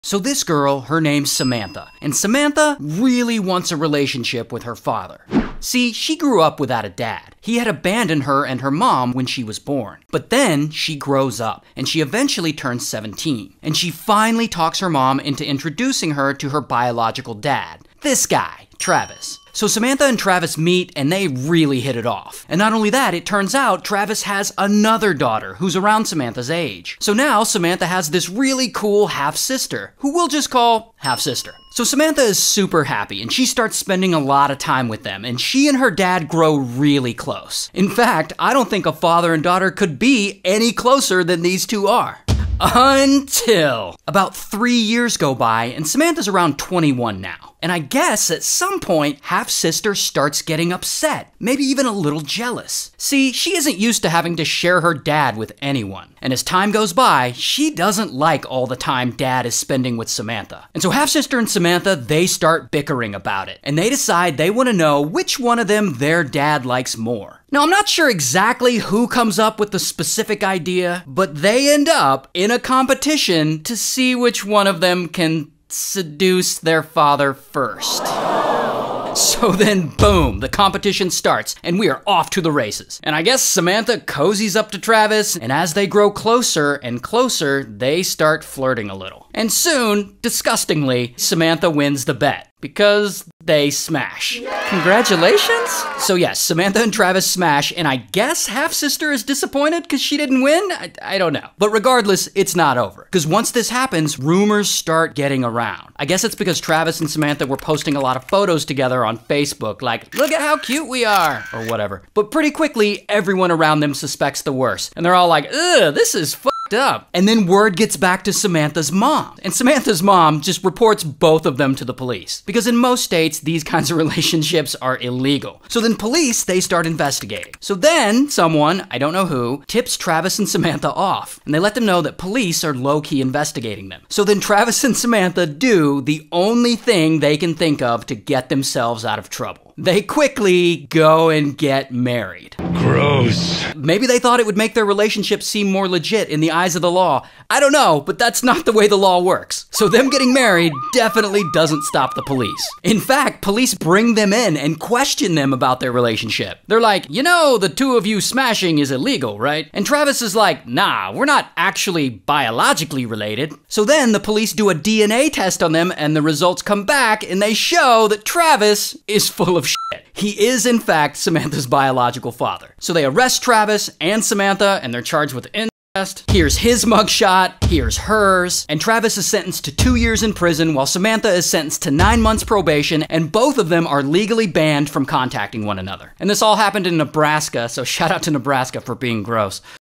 Remember her? She married her dad
This is the story of Travis Fieldgrove and Samantha Kershner